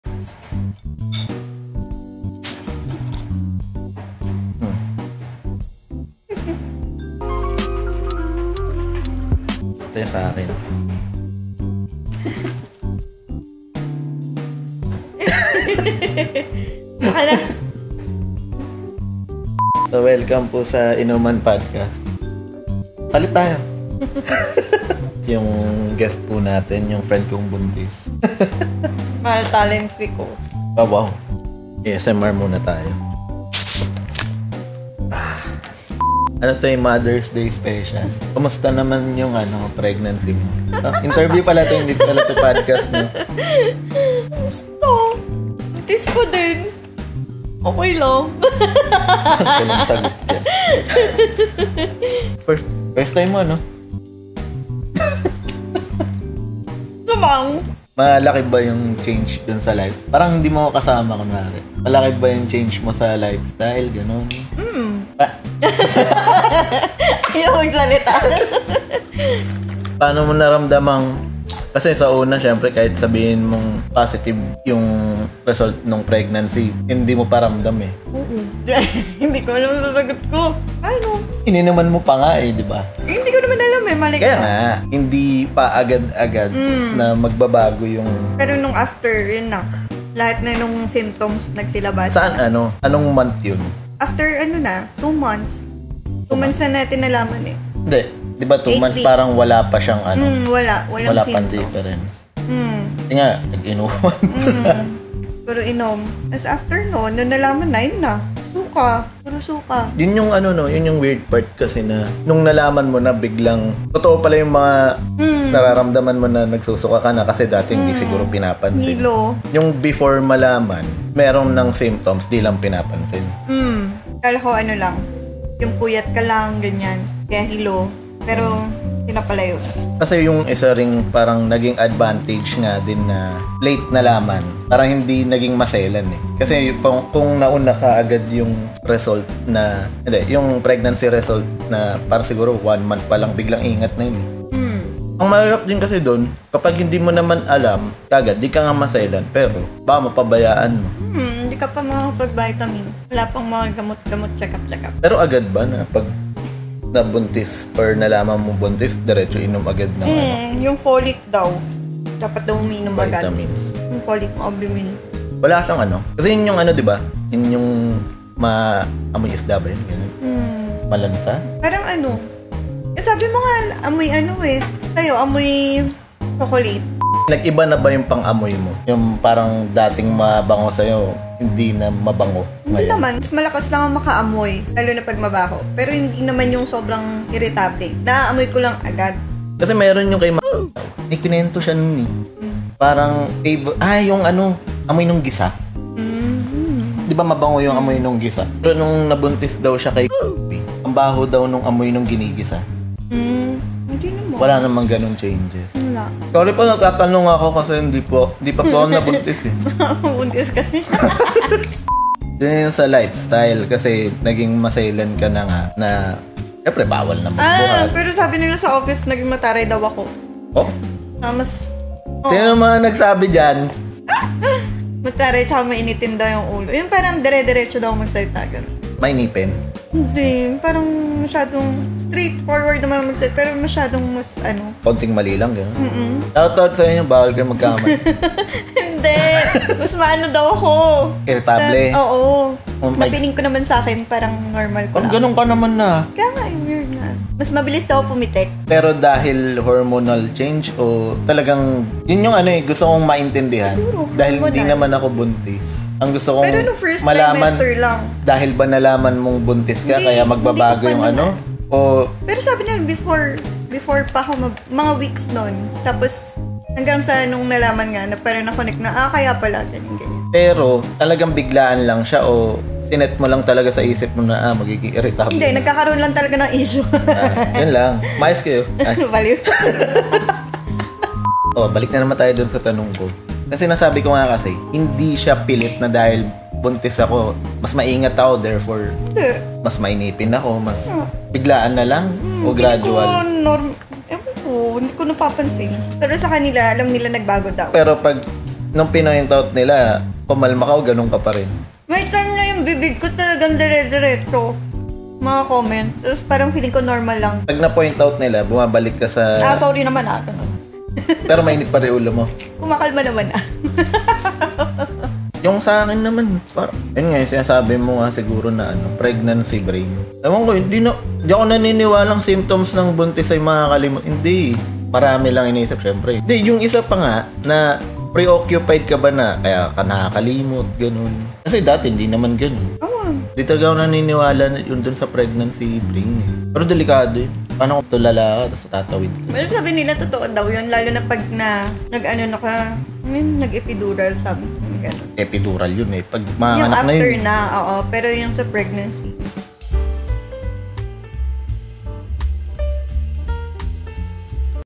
Mga Changes Sa Pagbubuntis | Inuman Podcast Bites
Welcome ulit sa Inuman Podcast Bites, ang good vibes podcast ng mga hindi Filipino vloggers habang nasa inuman session! 🍺🥃🎙️🤣

In this Inuman Podcast Bite, napag-usapan ng bestfriend couple ang about sa na-observe na mga pagbabago ni Vane nung nalaman niyang siya ay may dragon baby na.